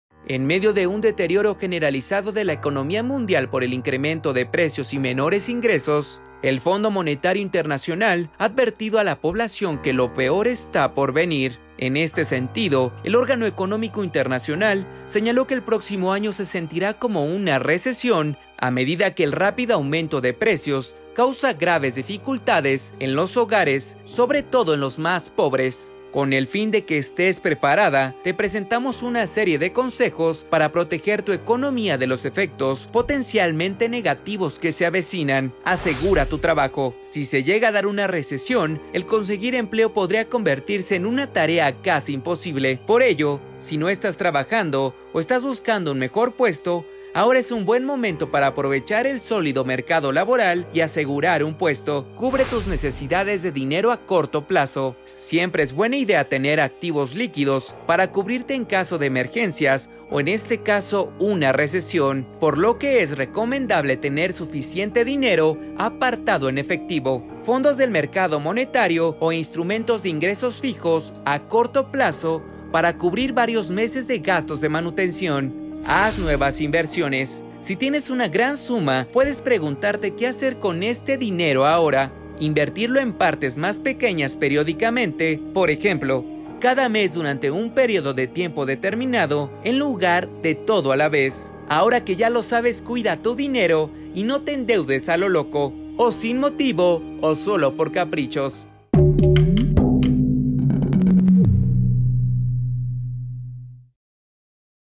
¿Lo peor está por venir? Consejos para proteger tu economía de una posible recesión
Economía mundial en medio de una posible crisis! El Fondo Monetario Internacional (FMI) ha advertido a la población que “lo peor está por venir”.

El órgano económico internacional señaló que el próximo año se sentirá como una recesión, a medida que el rápido aumento de precios causa “graves dificultades” en los hogares, sobre todo en los más pobres.

Es por ello que te recomendamos en este video una serie de consejos.